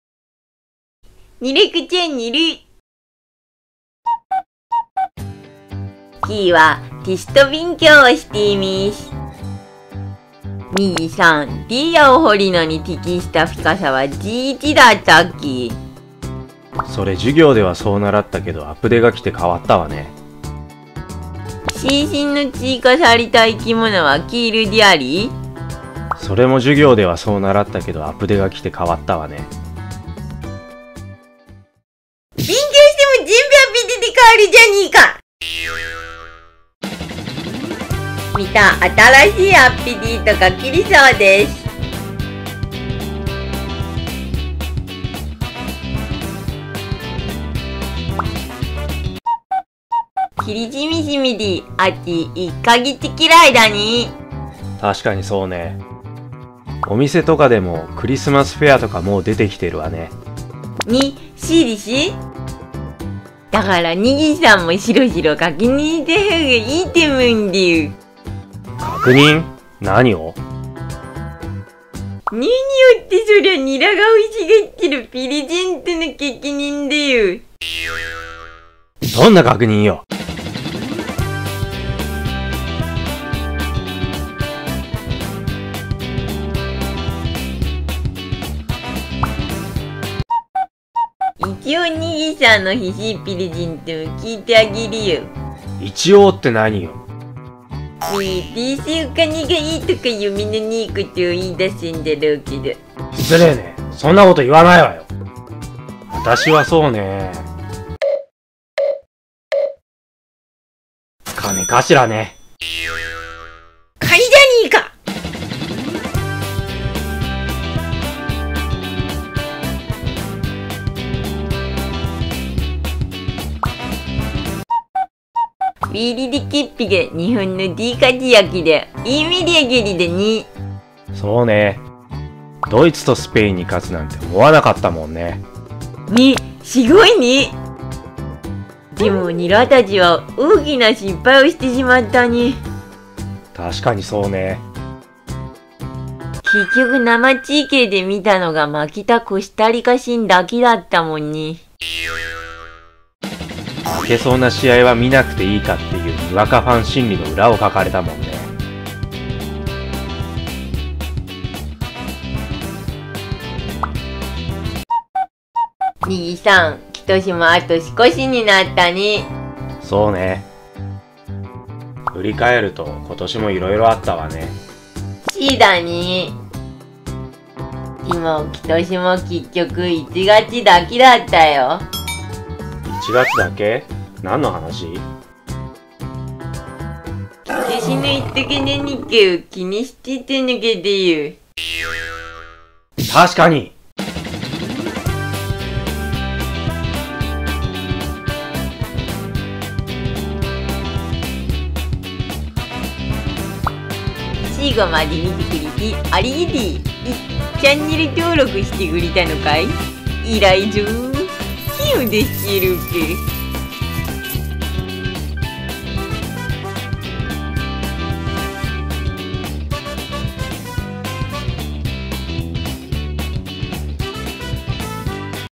【マイクラまんが】マイクラ学園まとめ３１話～３５話【ニラ子劇場】
ニラ子「まんがチャンニルも　よろしきにー(・皿・)/！」
根岸「見に来てねー」

□まんがチャンネル：マイクラまんがニラ子劇場

ニラ子：
口がイーーーってなっているから滑舌が良くない女のコ。
おだんご頭がチャームポイント。

根岸マツコ（にぎしさん）：
ニラ子の相棒。ときどき通訳。